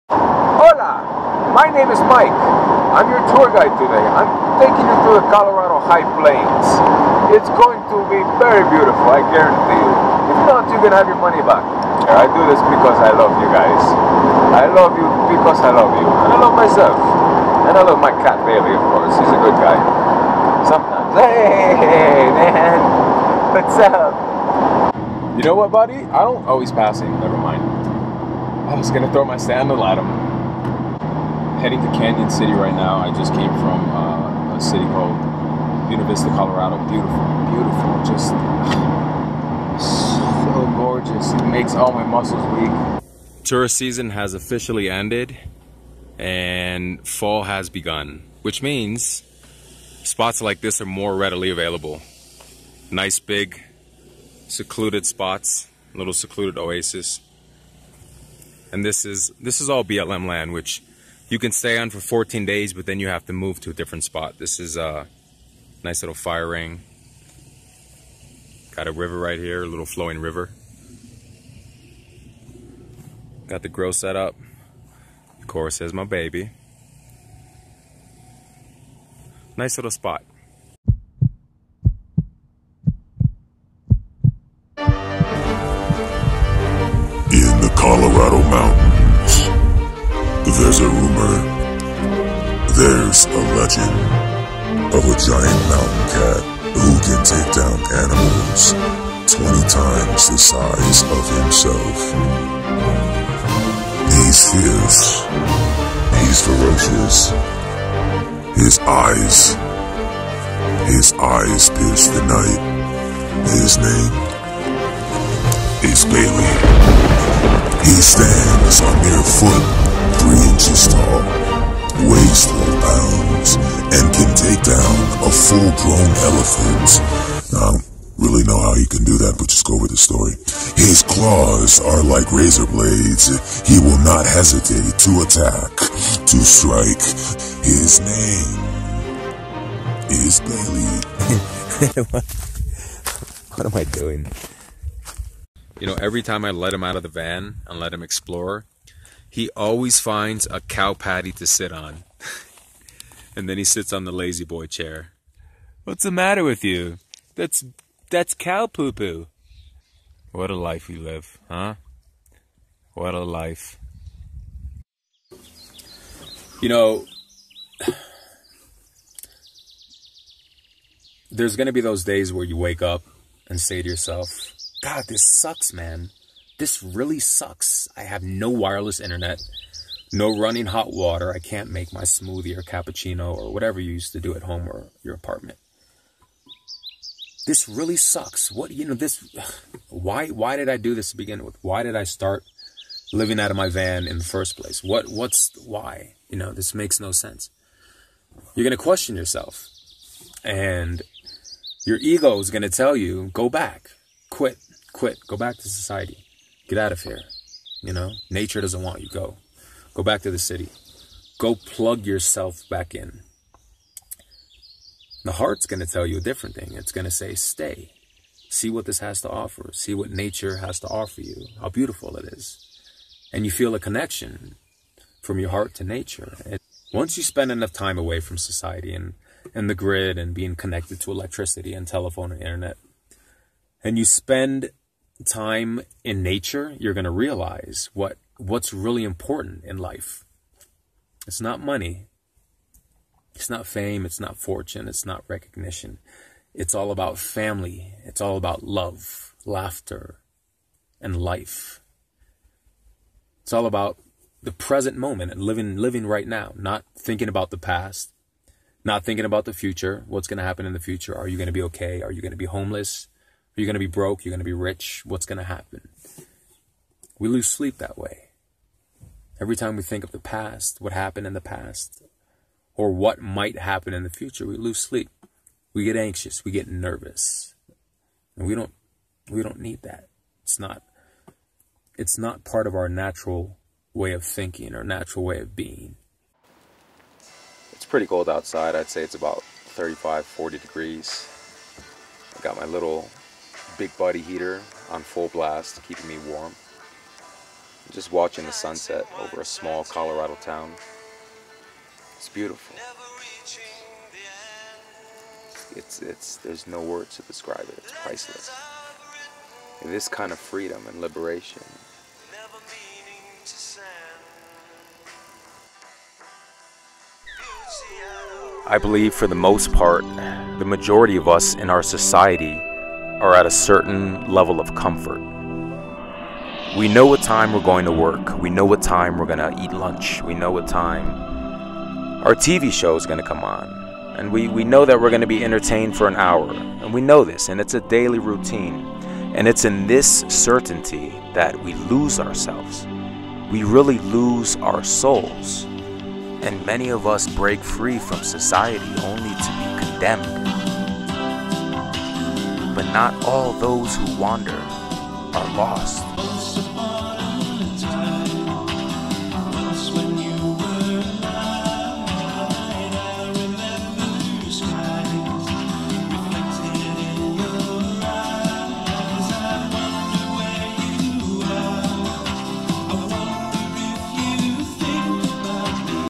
Living off the grid
My spectacular, wonderful, liberated people!! I love all of you. I believe that van dwelling has enriched my spirit and soul but there are drawbacks to this lifestyle if you choose to see them that way. Be safe and happy!